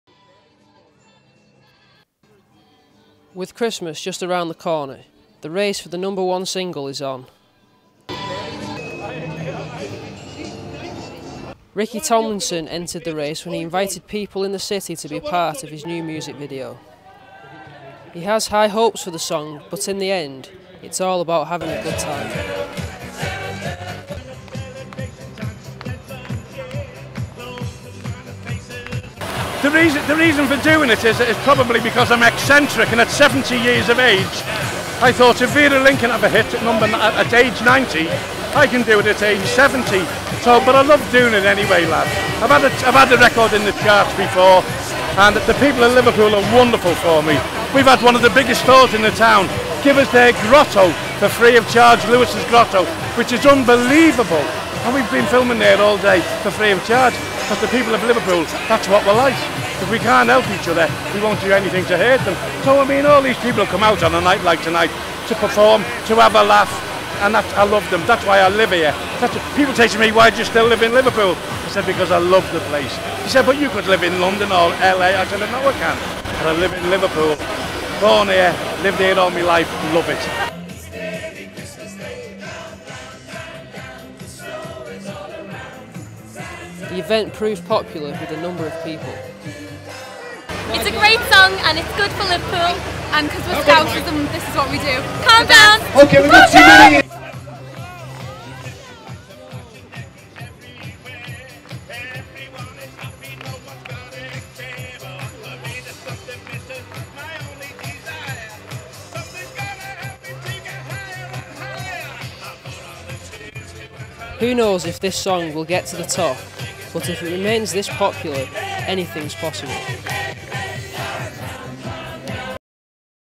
Ricky Tomlinson films Christmas single
Liverpool got into the Christmas spirit when Ricky Tomlinson invited members of the public to be a part of his new music video